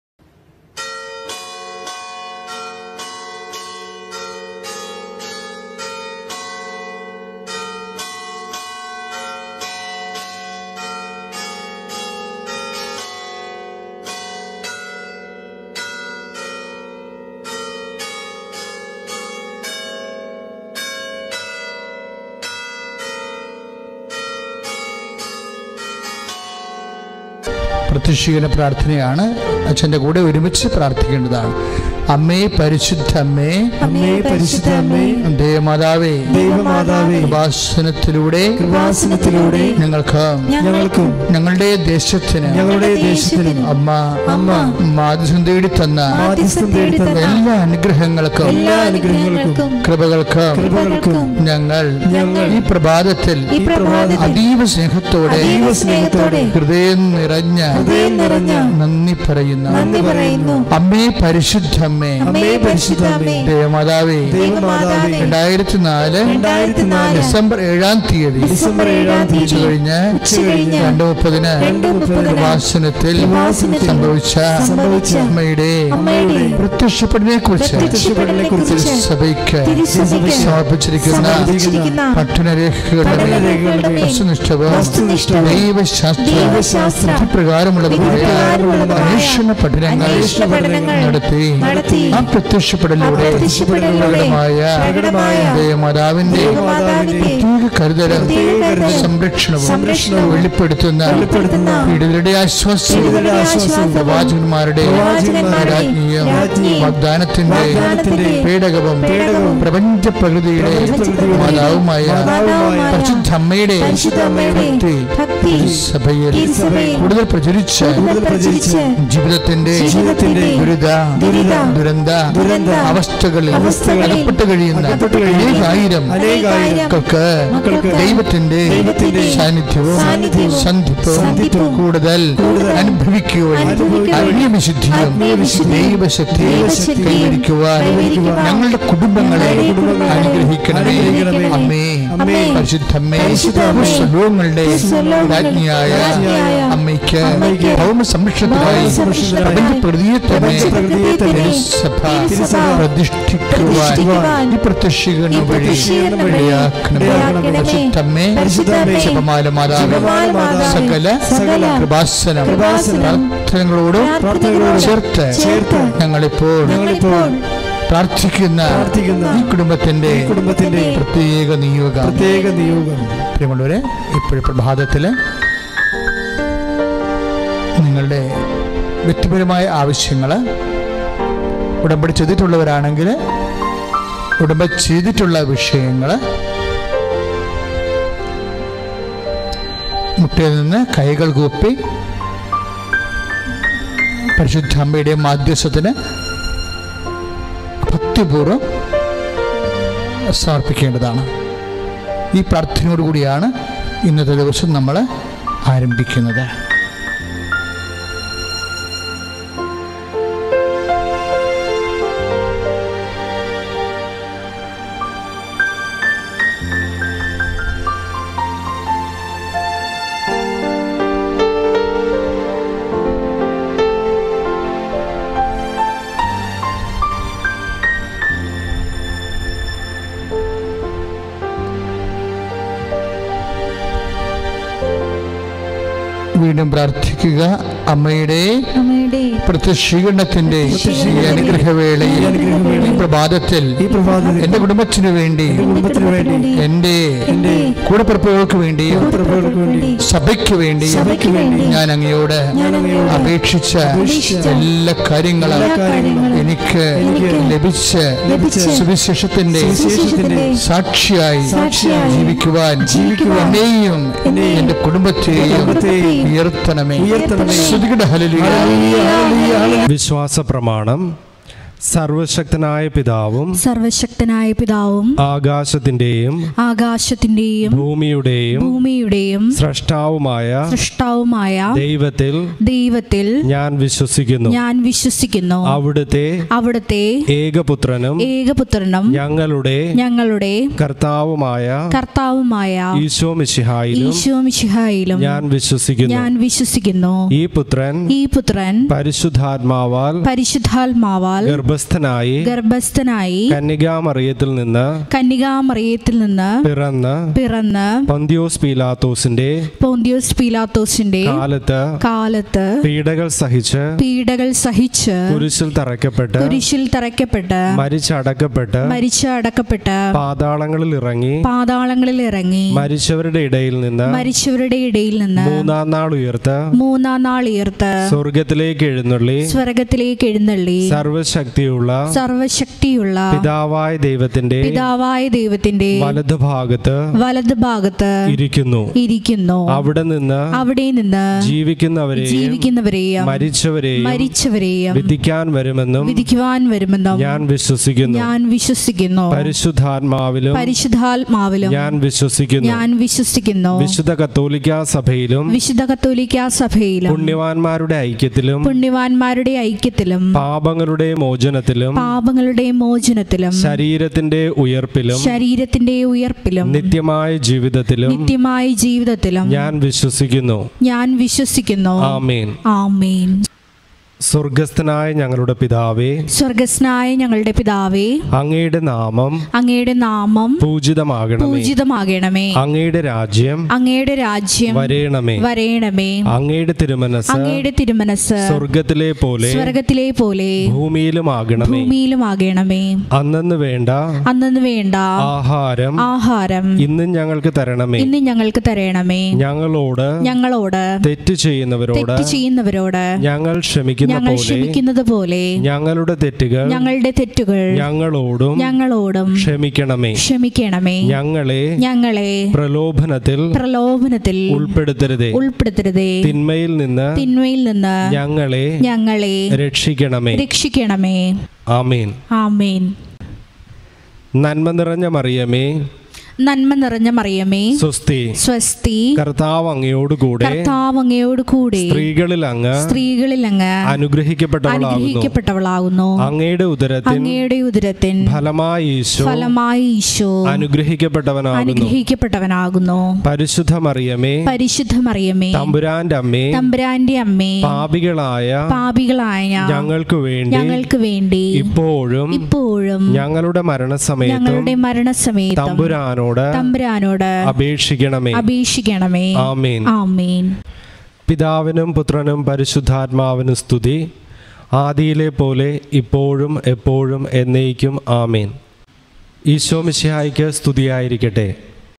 പ്രത്യക്ഷീകരണമദ്ധ്യസ്ഥപ്രാർത്ഥന
ഉടമ്പടി എടുക്കാത്തവർ ഈ പ്രാർത്ഥന ചൊല്ലിക്കഴിഞ്ഞ് ഇന്ന് തന്നെ ഉടമ്പടി എടുത്ത് അമ്മമാതാവിൻറെ അനുഗ്രഹം പ്രാപിക്കുക!!